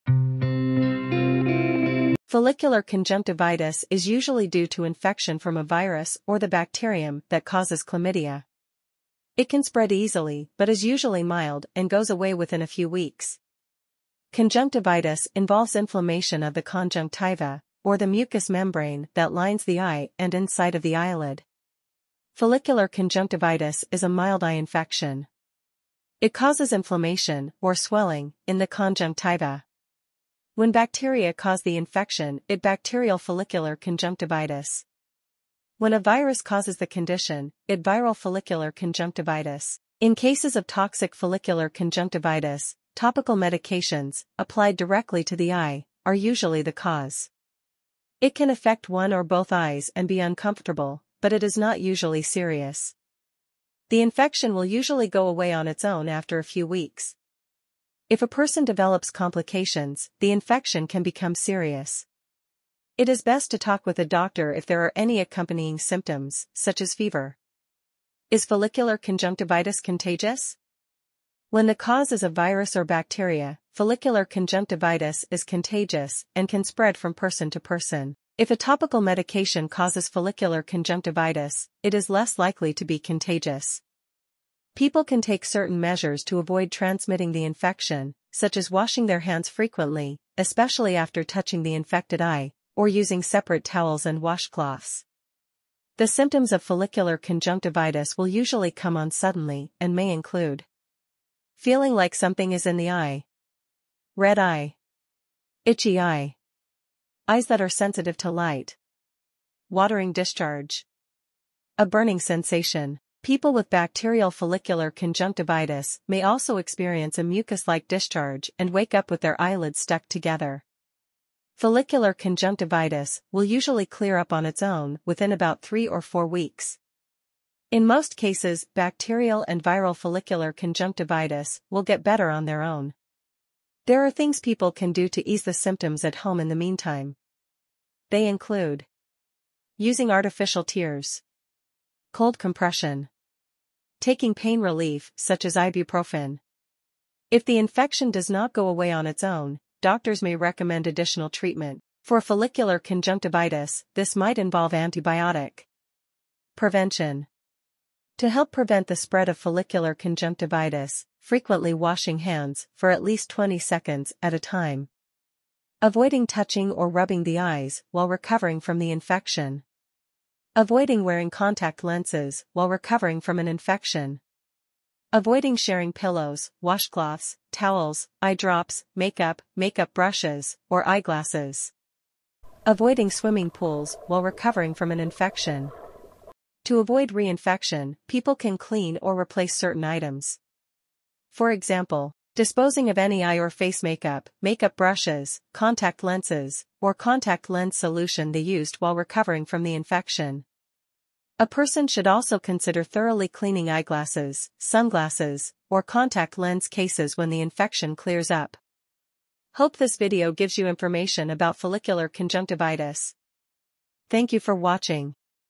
Follicular conjunctivitis | symptoms|Treatment|prevention|Season of follicular conjunctivitis 2024
learn what is follicular conjunctivitis with symptoms cause treatment and prevention.

season of follicular conjunctivitis is coming soon, so take care of your eyes and watch this video to prevent it.

hope this video gives you information about follicular conjunctivitis

what is follicular conjunctivitis?
Follicular conjunctivitis
Seasonal Conjunctivitis
Pink eye